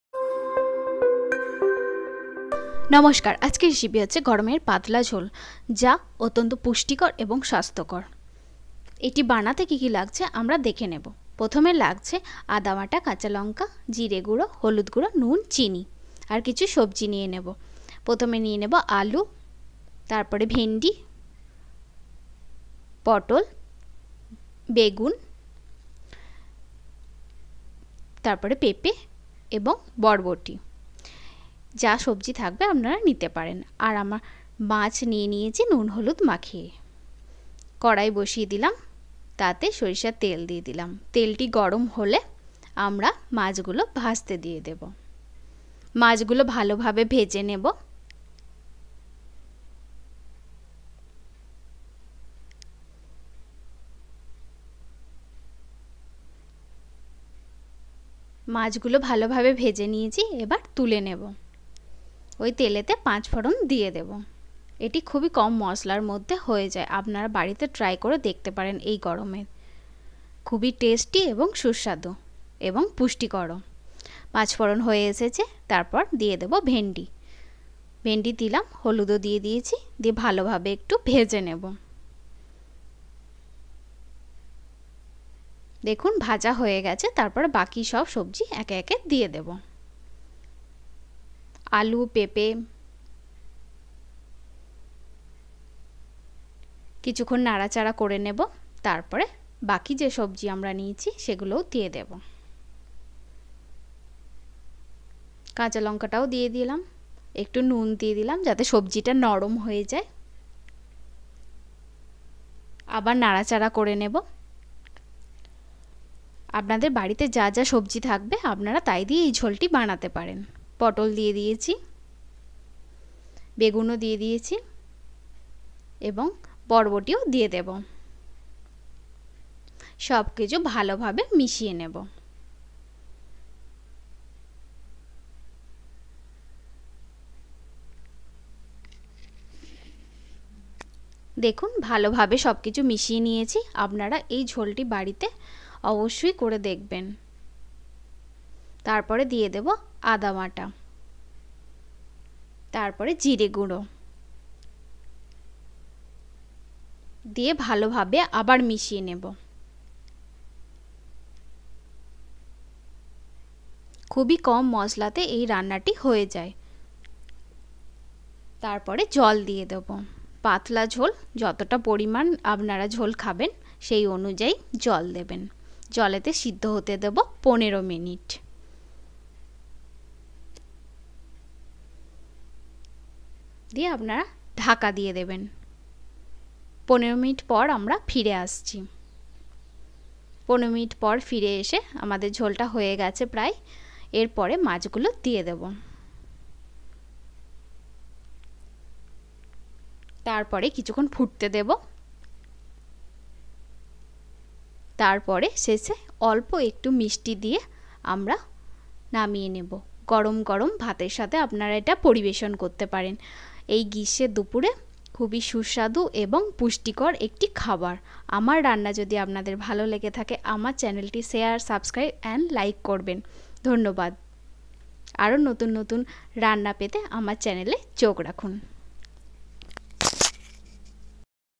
গ্রীষ্মের দুপুরে কম তেল মশলায় এই পাতলা ঝোল স্বাস্থ্যের জন্য খুবই উপকারী //Fish and Sobji Jhol..
sobjir Jhol || Summer Special Recipe || Bengali Recipe || Fish And Sobji Jhol ||